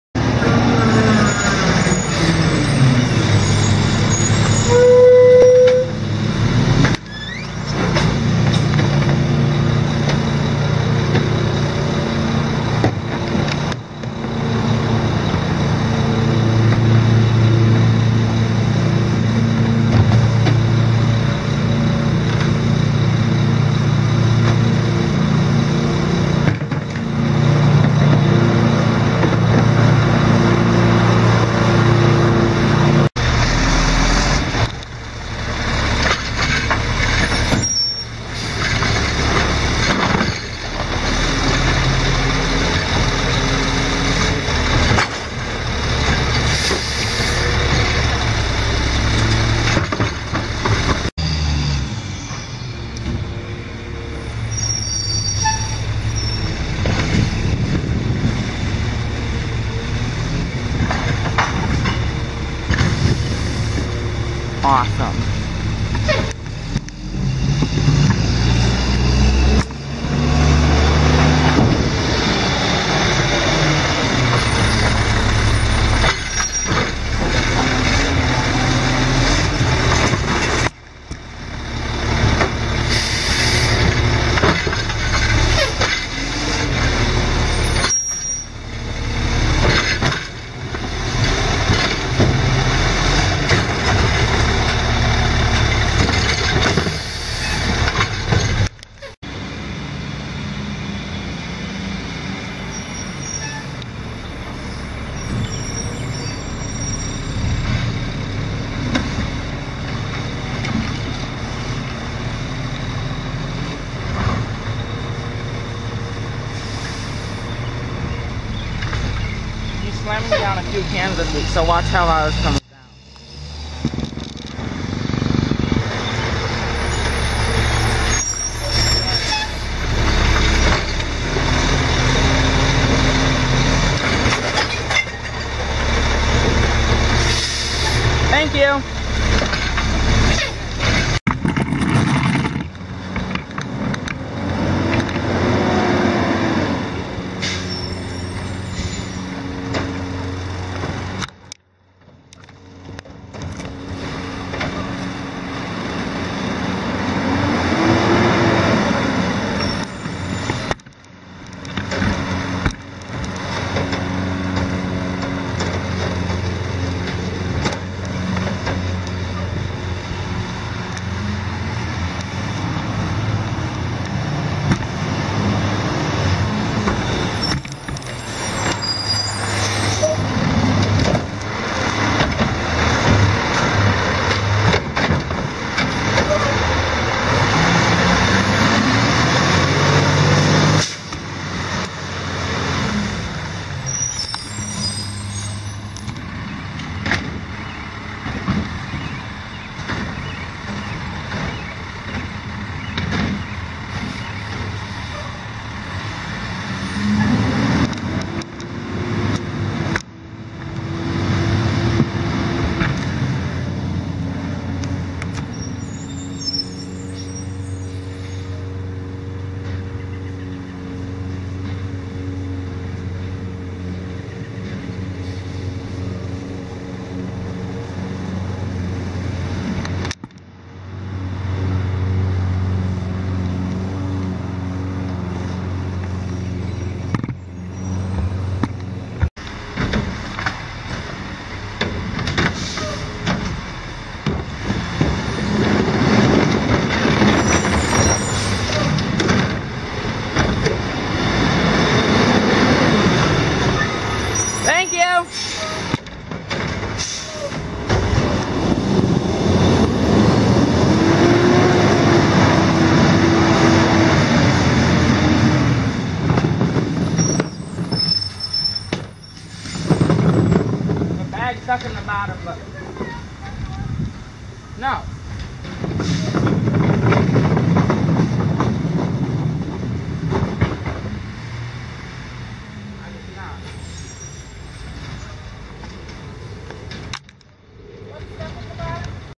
My yard waste, recycle and trash on 6/27/14 (tripod footage)
This is my first tripod footage.  These are the trucks that are currently picking up in my neighborhood:  Mack LE McNeilus rear loader on yard waste, Mack LE McNeilus M/A collecting recycling and Autocar McNeilus with Curotto Slammin' Eagle doing garbage.  Sorry if the audio and video are not at the same speed.

That was my first time helping the yard waste driver, so that it is why I had some trouble.

50th video special coming up soon!!!

WM please don't use this video to get the drivers in trouble.  This video is solely for the entertainment of other YouTubers.